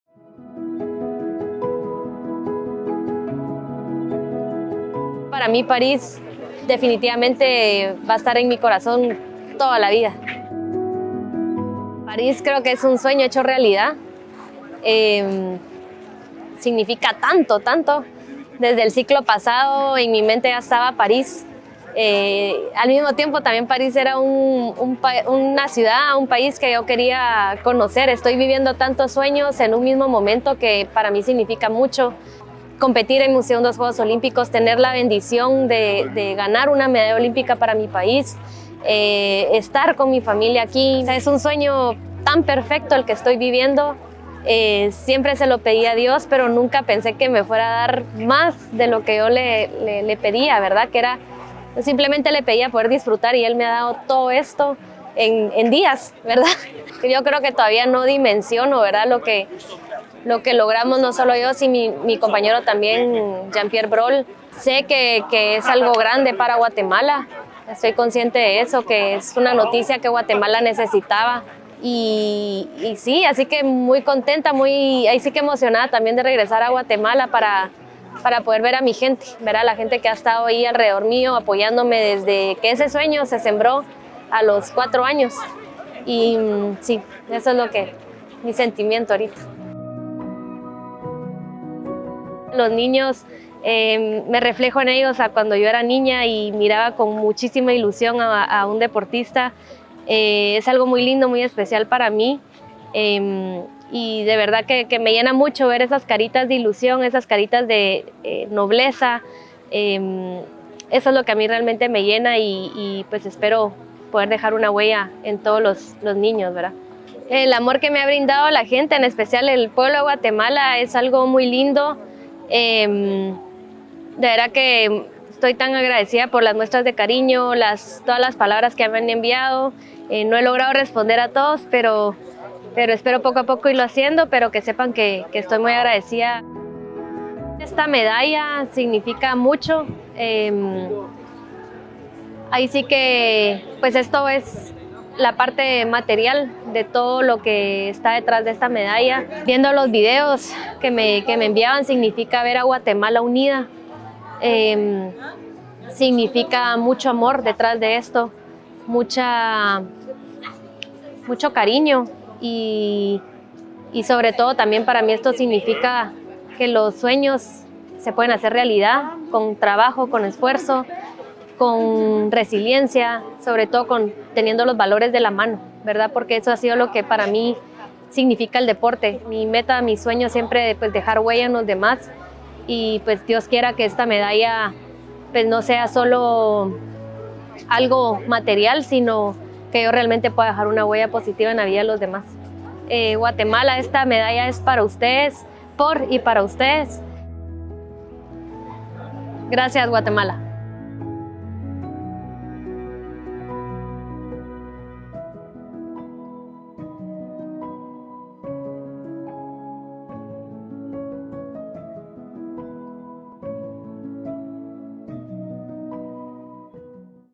ADRIANA RUANO y el significado que hay detrás de la histórica medalla de oro en Paris2024 🥇💙
🥇💙 Desde su propia voz: Adriana Ruano y el significado que hay detrás de la histórica medalla de oro en Paris2024.